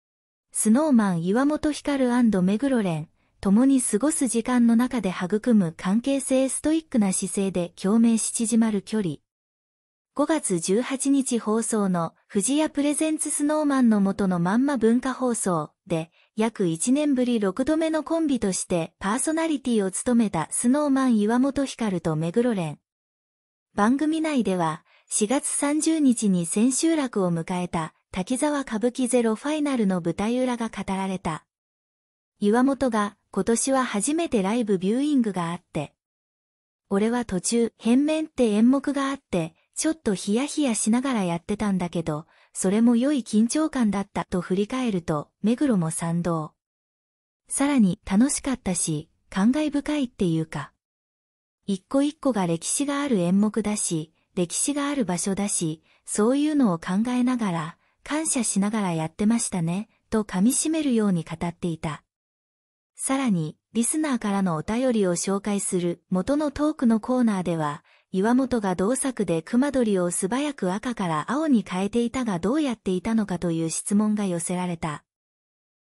Snow Man 岩本照＆目黒蓮、共に過ごす時間の中で育む関係性 ストイックな姿勢で共鳴し縮まる距離
5月18日放送の『不二家 presents Snow Manの素のまんま』（文化放送）で、約1年ぶり6度目のコンビとしてパーソナリティを務めたSnow Man・岩本照と目黒蓮。番組内では、4月30日に千秋楽を迎えた『滝沢歌舞伎ZERO FINAL』の舞台裏が語られた。　岩本が「今年は初めてライブビューイングがあって。俺は途中『変面』って演目があって、ちょっとヒヤヒヤしながらやってたんだけど、それも良い緊張感だった」と振り返ると、目黒も賛同。さらに「楽しかったし、感慨深いっていうか。一個一個が歴史がある演目だし、歴史がある場所だし、そういうのを考えながら、

IMPORTANT!